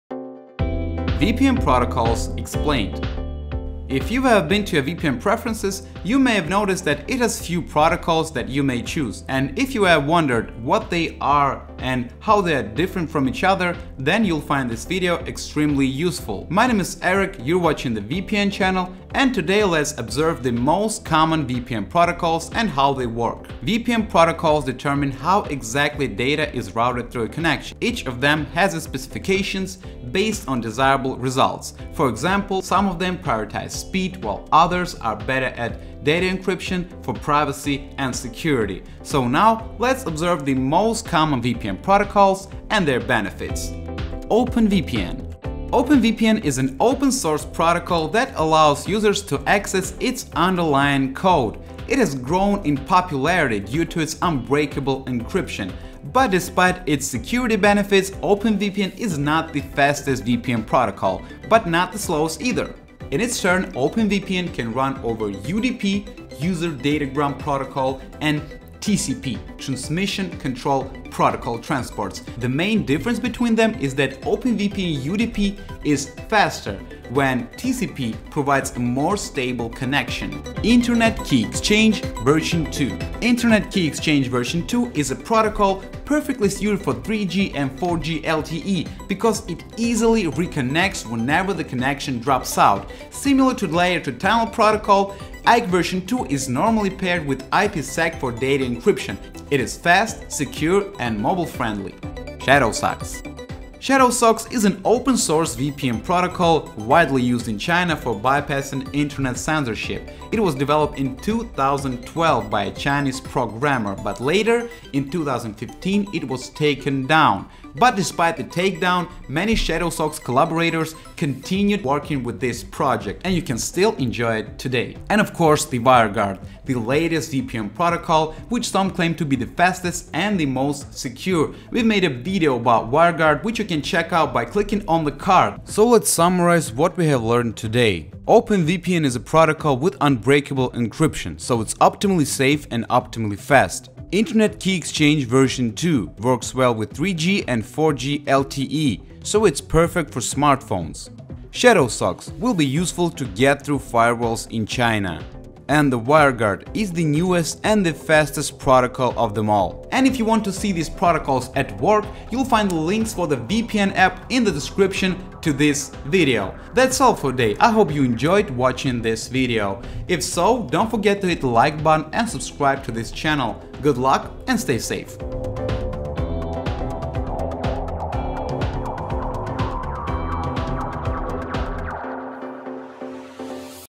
VPN Protocols Explained (OpenVPN, IKEv2, Shadowsocks, Wireguard)
In this video we explain how VPN protocols work, what are the differences between them and what are their advantages in simple words. Learn how to work with OpenVPN, IKEv2, Shadowsocks or Wiregurad and how to choose them depending on your needs.